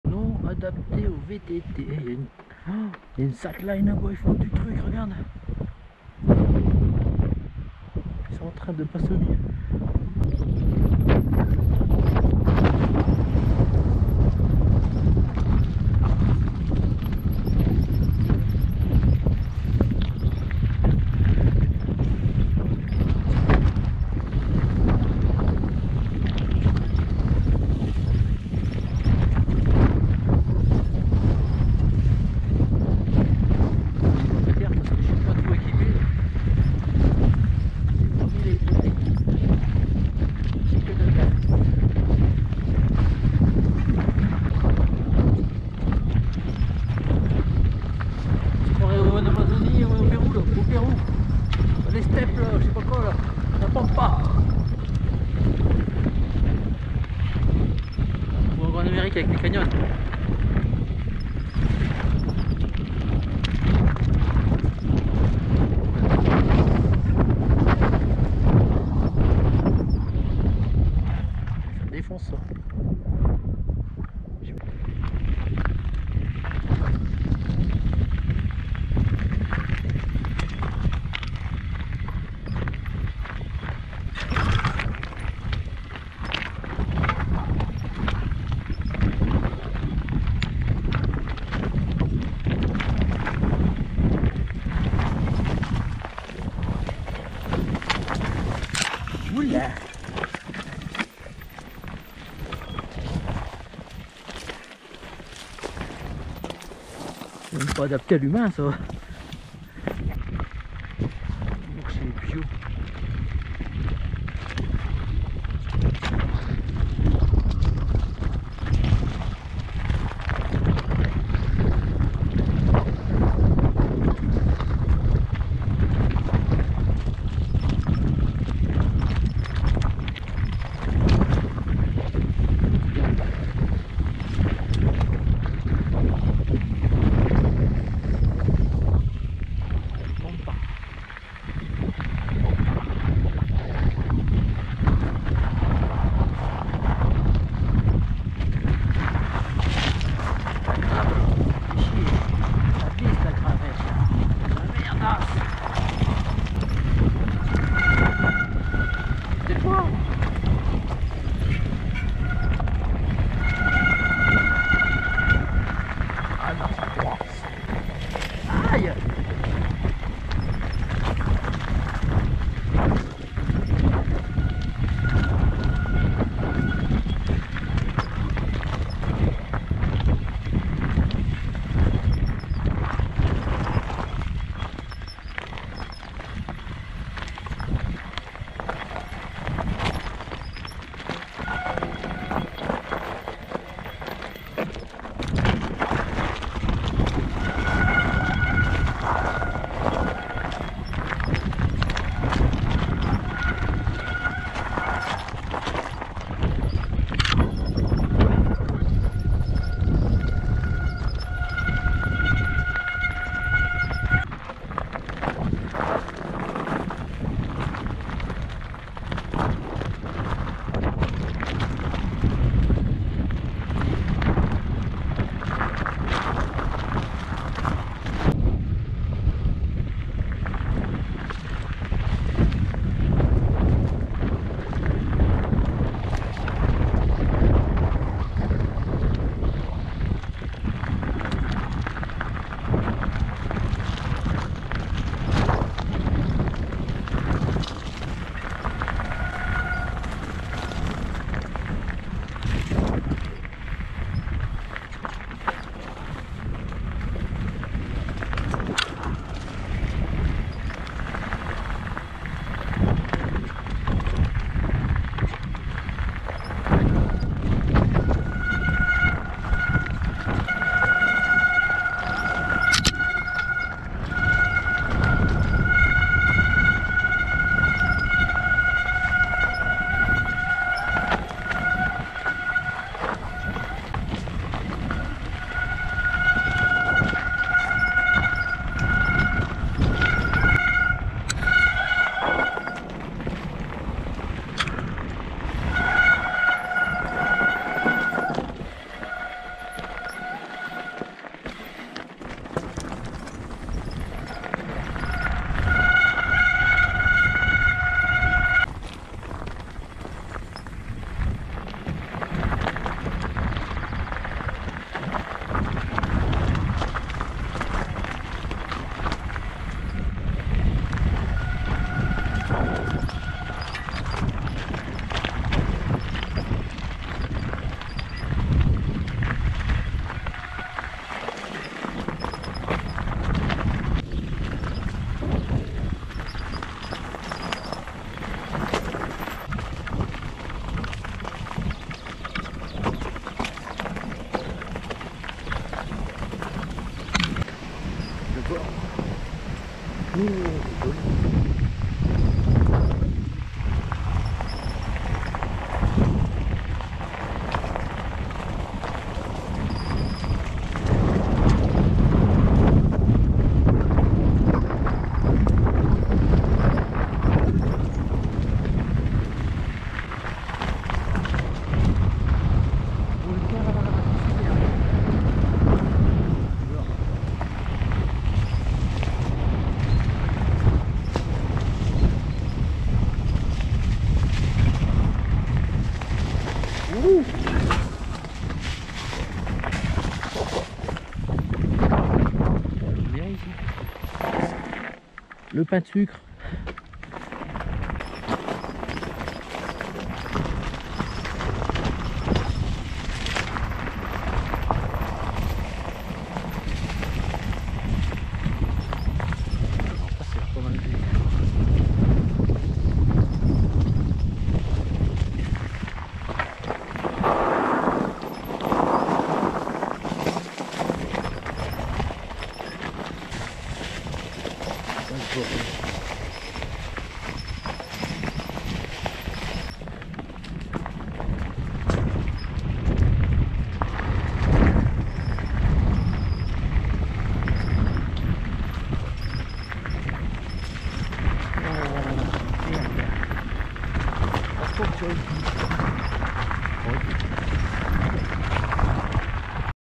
vtt gorges du verdon single cadieres de brandis
single sympa mais glissant avec les gravettes au debut , tres belle vue sur cadieres de brandis , il y avait des funambules sur le sommet !! au depart panneau " ^ NON ADAPTE AU VTT  " ca passe facile , mais peut etre que ca agace les habitants du village de voir trop de velo ? mefier vous des pieges vtt ,clous et autres cables , on ne sait jamais !! par contre ils y a pas mal de ronces par endroits !!

vtt velo enduro dh e bike elec electrique xc trail freeride street dirt mtb park redbull rampage downhill vosges alsace france